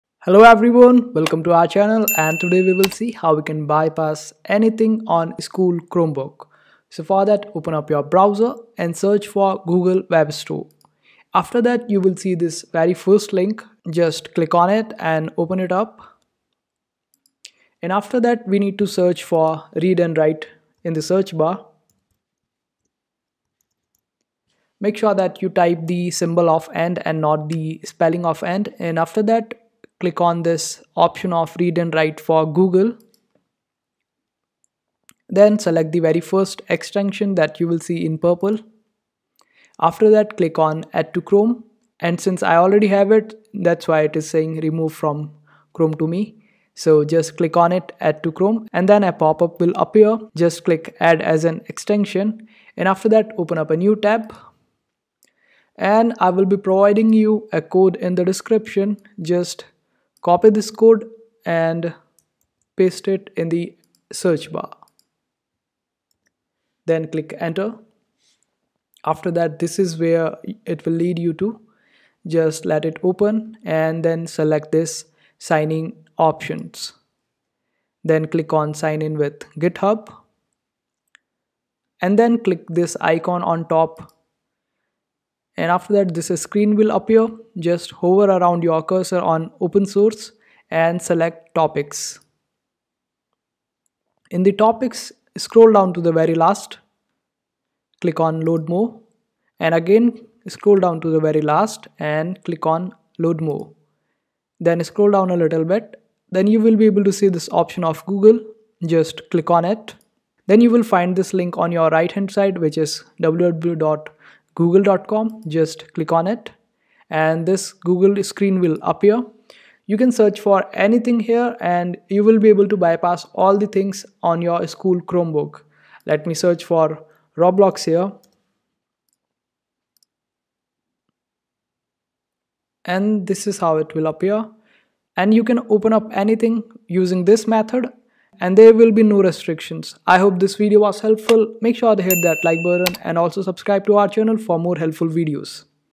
How To Bypass On School Chromebook (SIMPLE!)
How To Bypass On School Chromebook . It’s a very easy tutorial, I will explain everything to you step by step.